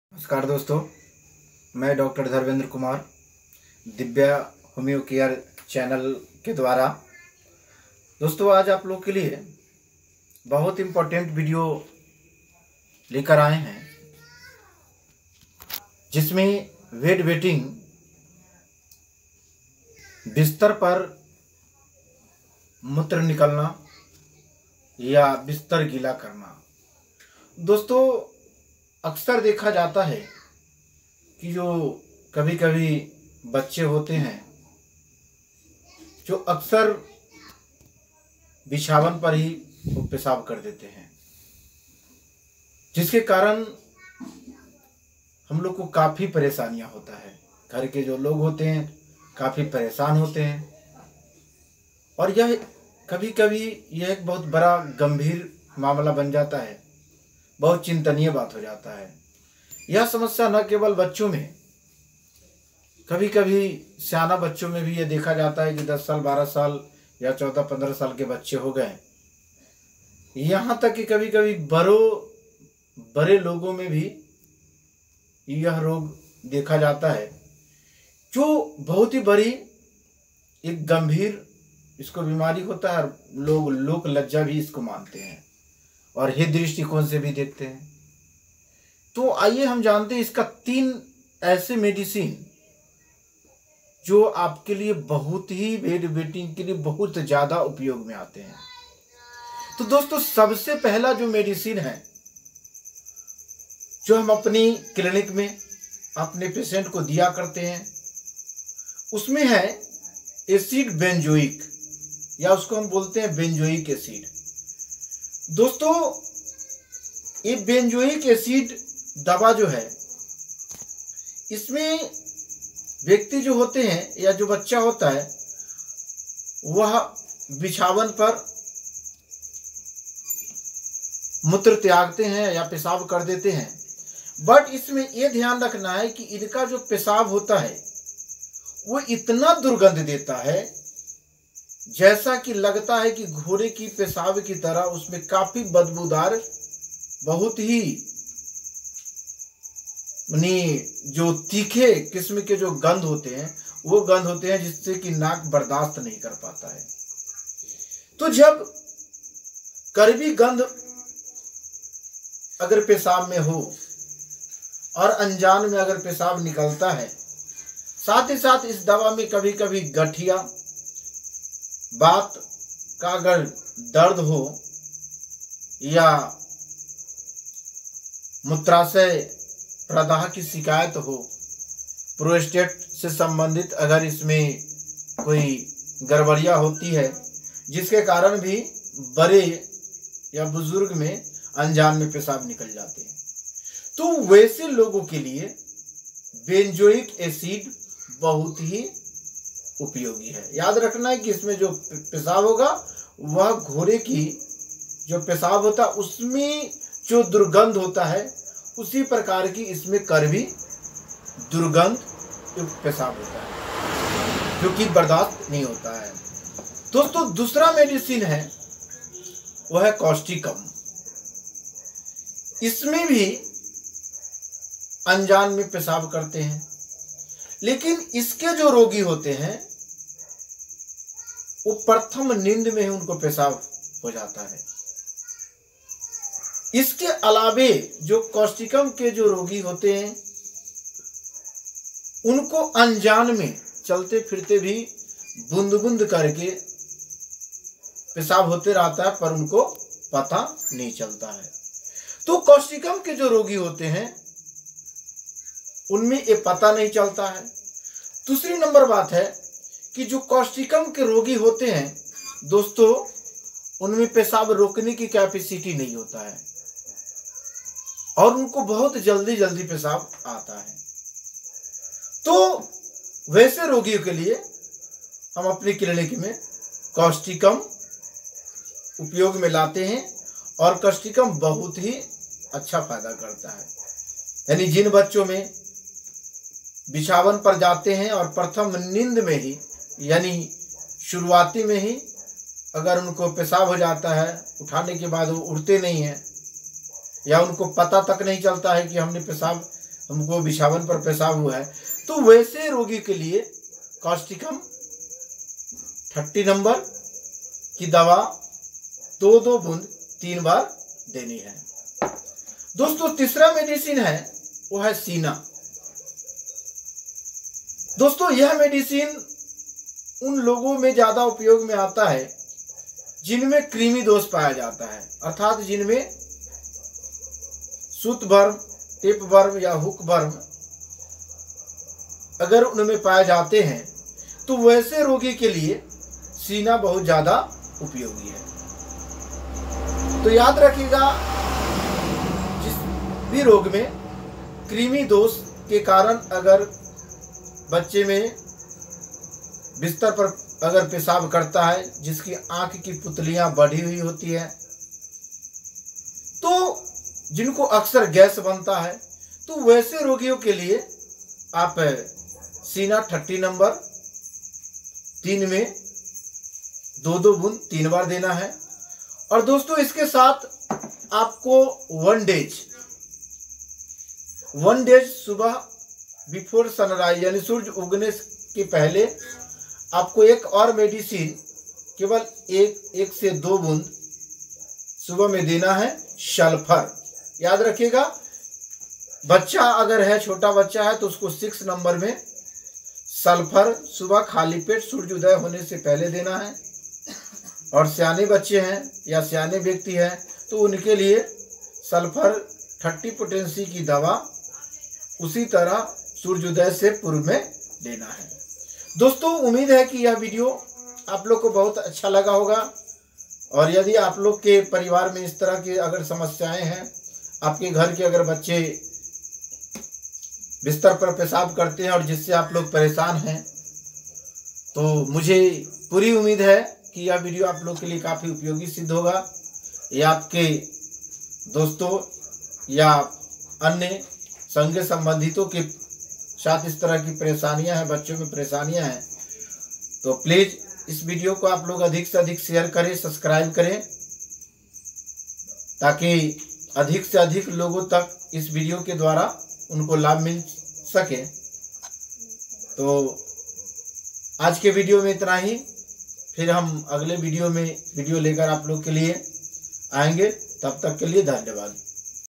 Bed weting discharge urination homeopathic medicine Acid benzoic
This video gives many symptoms and cure in homoeopathic medicine for children who discharge urine on bed or Bed weting#discharge urination#homeopathic #medicine#Acid benzoic#CINA#CAUSTICUM#USE#.